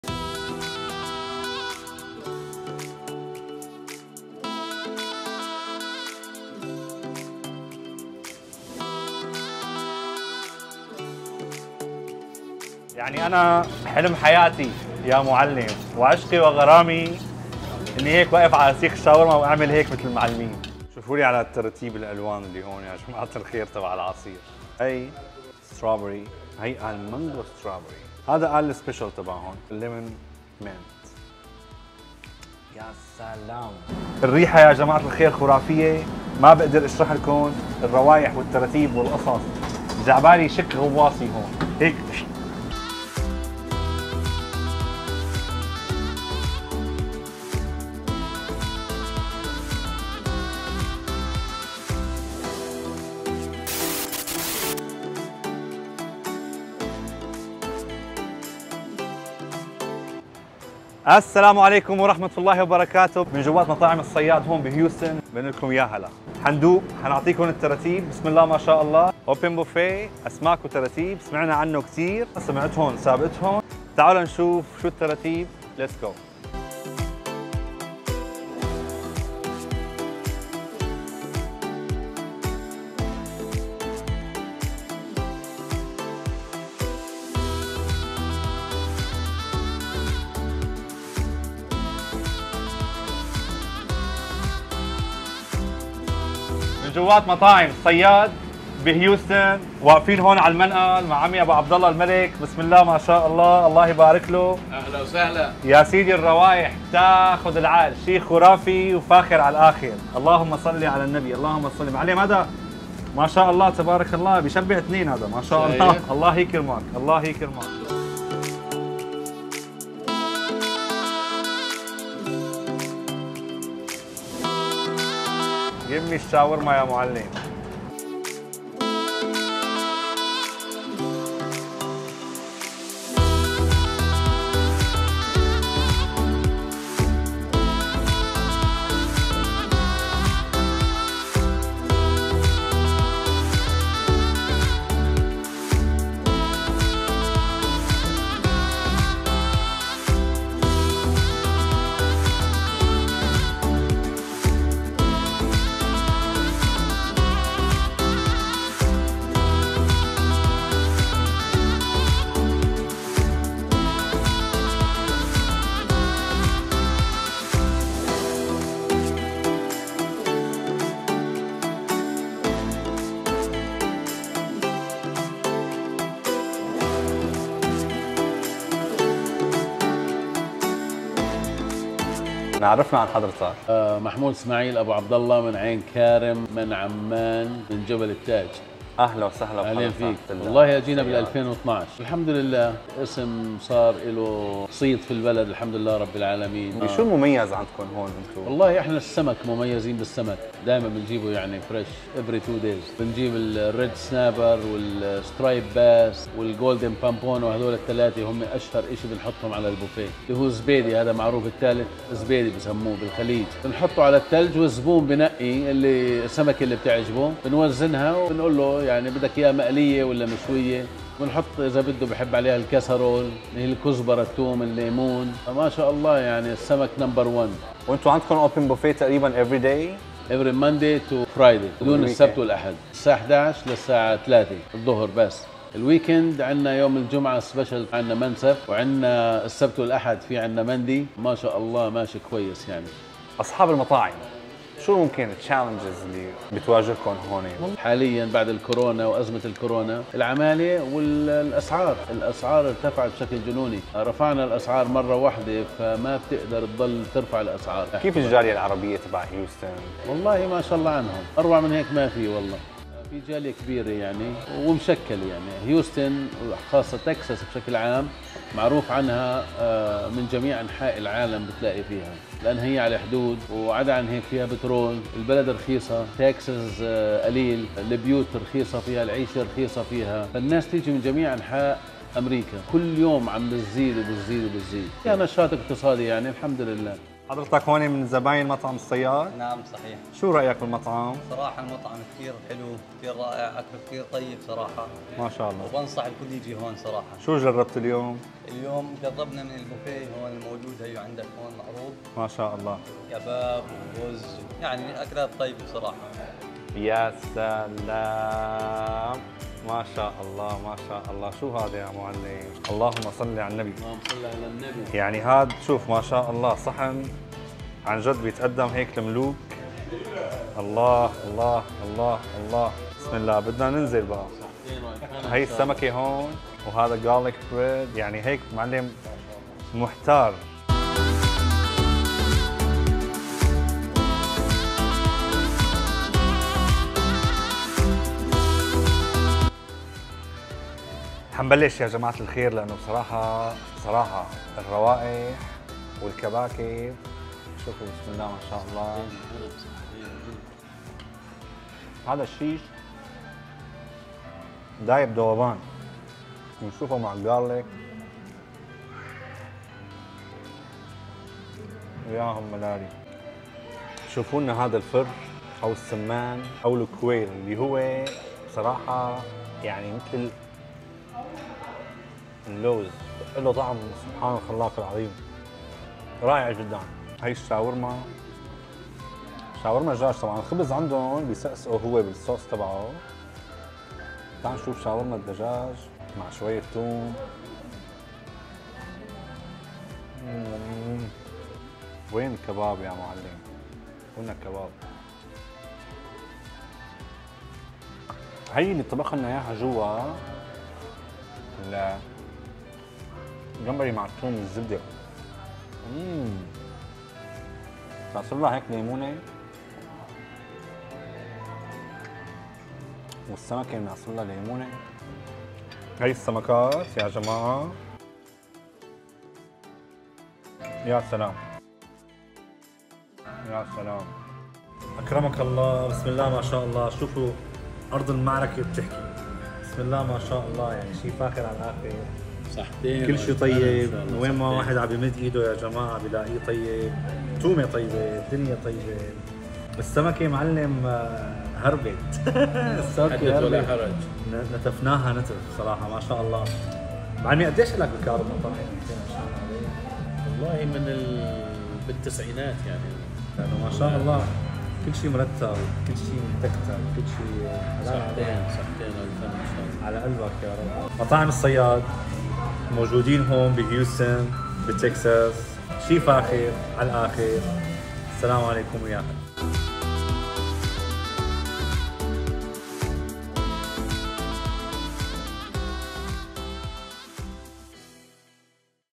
مأكولات بحرية في هيوستن عند مطعم الصياد
من هيوستن تكساس بنقلكم يا هلا

مطعم الصياد واحد من أفضل وأطيب المطاعم العربية في مدينة هيوستن ، يتميز بتقديم المأكولات البحرية الطازجة وخاصةً الأسماك ،بالإضافة إلى تشكيلة واسعة من المشاوي العربية على الفحم الطبيعي . يحتوي على بوفيه مفتوح من مختلف الأصناف وكذلك بإمكانك الطلب من المنيو.
تجربة جميلة جدا في مطعم الصياد , ينصح بتجربته 👍
العنوان